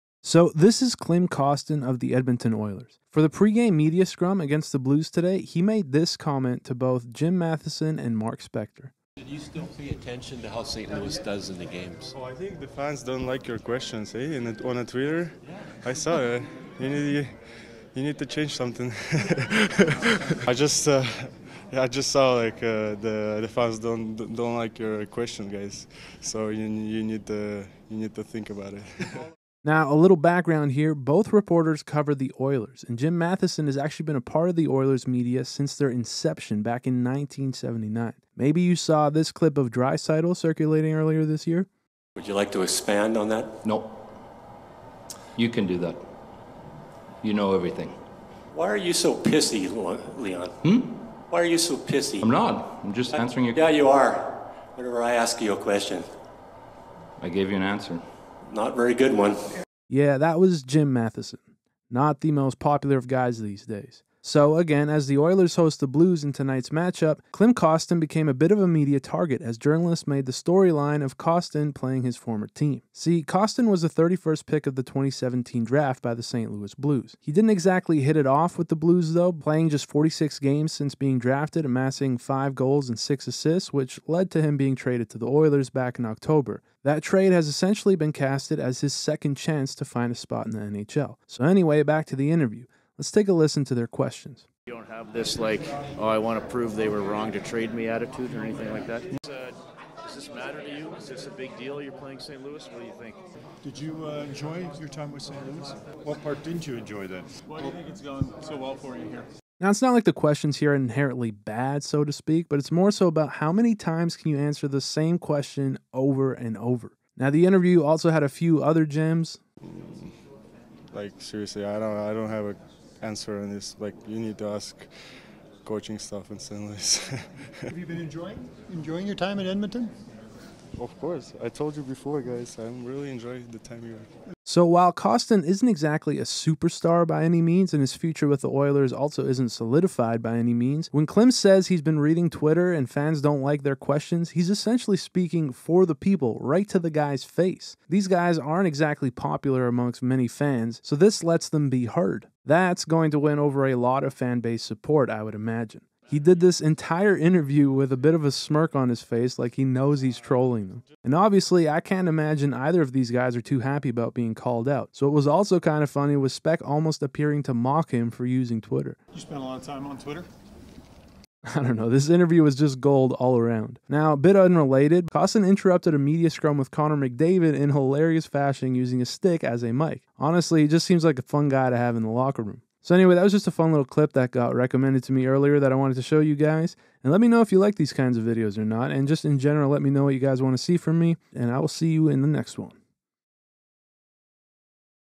THIS is how you win over a fan base
So in the lead up to tonights Edmonton Oilers vs St. Louis Blues game, much was made of Kostin playing his former team, the team that drafted him in 2017. What resulted in the media scrum, was that Klim Kostin had a hilarious pre game interview letting Jim Matheson and Mark Spector know what fans on Twitter think of them saying "I don't think fans like your questions, eh?" Now Jim Matheson's run in with Leon Draisaitl earlier this year solidified how many Oilers fans feel of him, so Kostin letting them know to their face how fans feel, is exactly the kind of stuff it takes to win over an entire fan base!